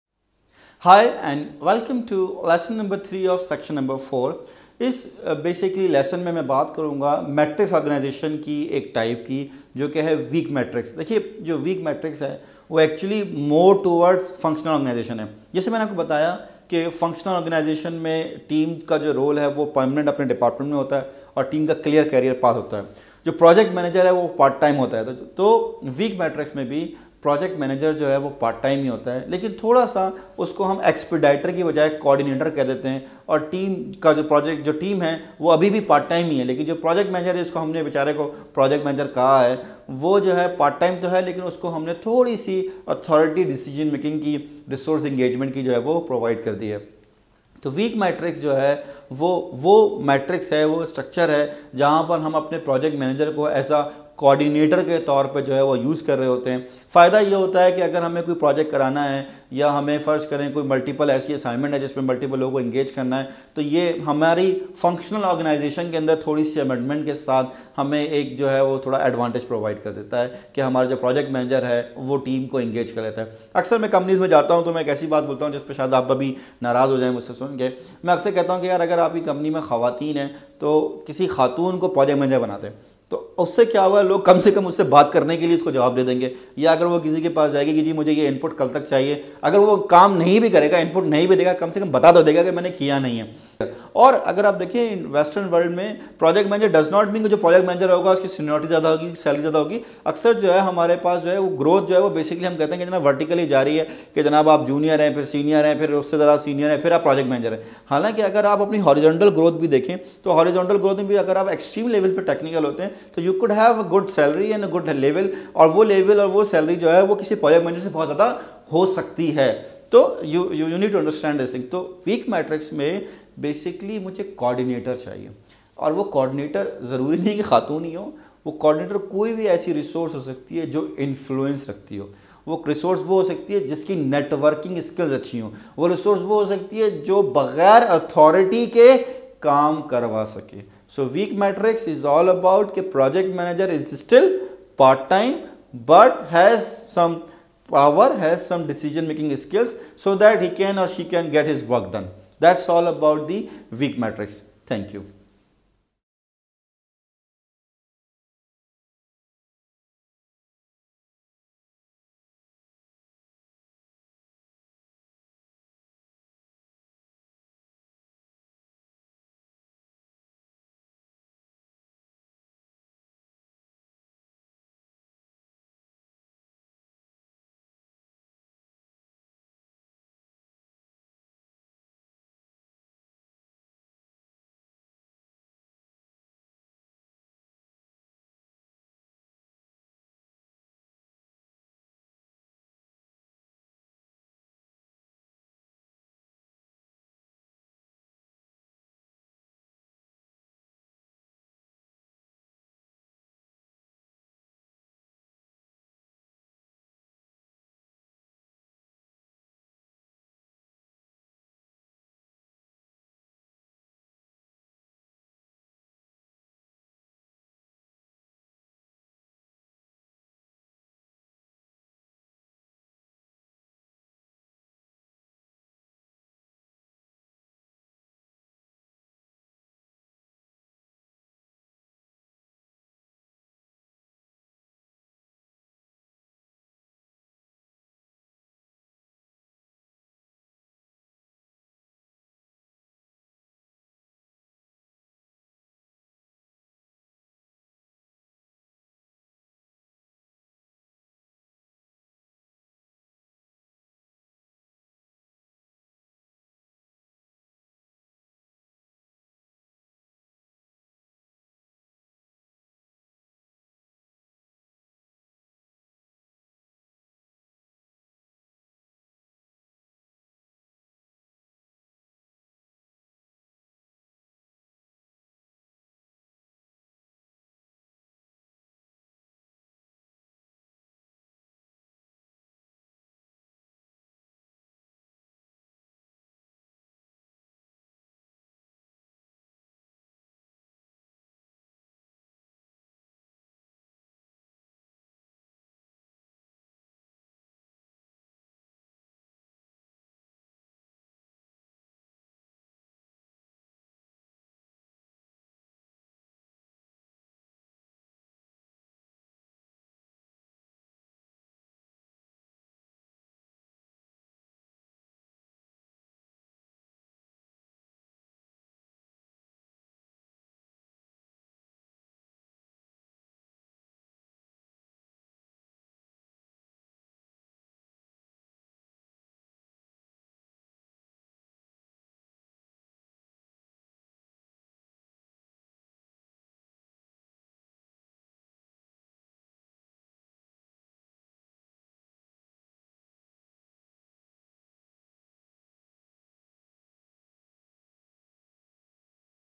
Weak Matrix Organizations | PMP® - PMI Exam Training Full course | AUC | Hindi Urdu
In this video, we'll be discussing the impact of weak matrix organizations on your career. We'll cover the definition of a weak matrix organization, the consequences of working in a weak matrix organization, and ways to break out of a weak matrix organization.

If you're working in a weak matrix organization, then you know that it can have a negative impact on your career. In this video, we'll provide you with the information you need to understand the consequences of working in a weak matrix organization and show you how to break free from it. With this knowledge, you can start building a successful career that doesn't rely on a weak matrix organization!